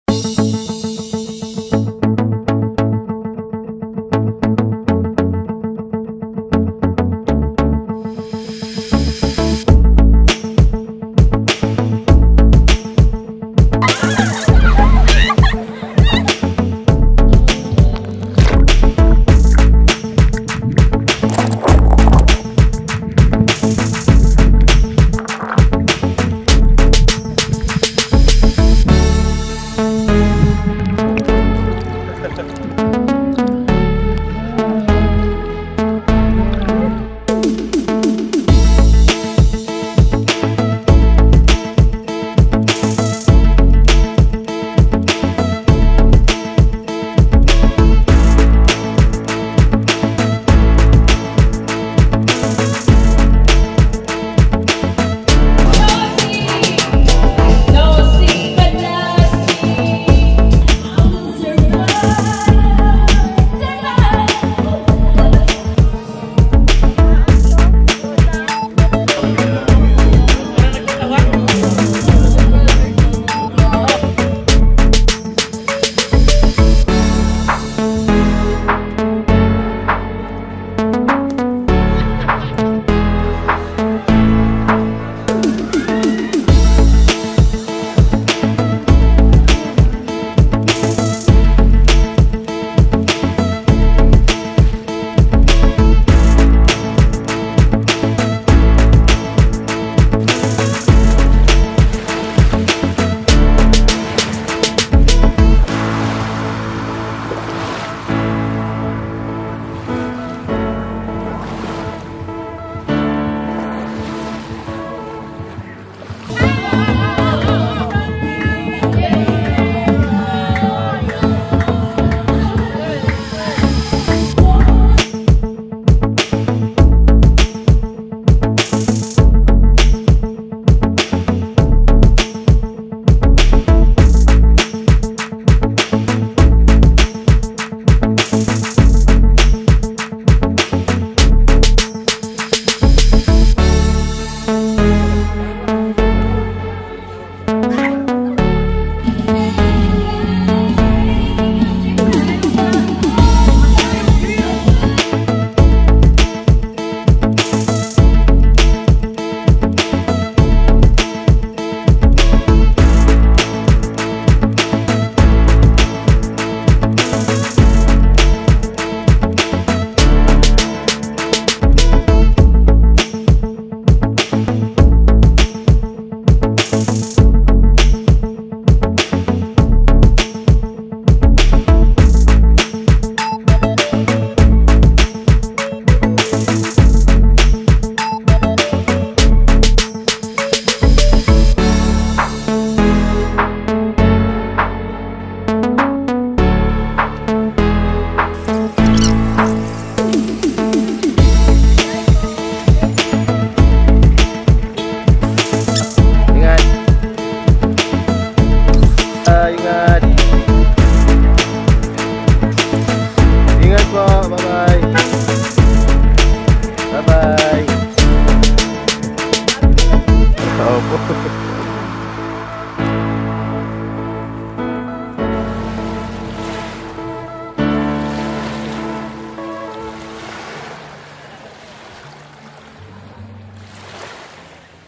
Sable Blanc (Sandy White) Beach Resort - Lobo, Batangas 2023
We celebrated my father-in-law's 60th birthday in here.
3 days, 2 nights.
Family, sandy beach, clear water, very accommodating staff and owner = GREAT EXPERIENCE!
Sable Blanc Beach Resort - CERTIFIED ASTIG!
Highly recommended!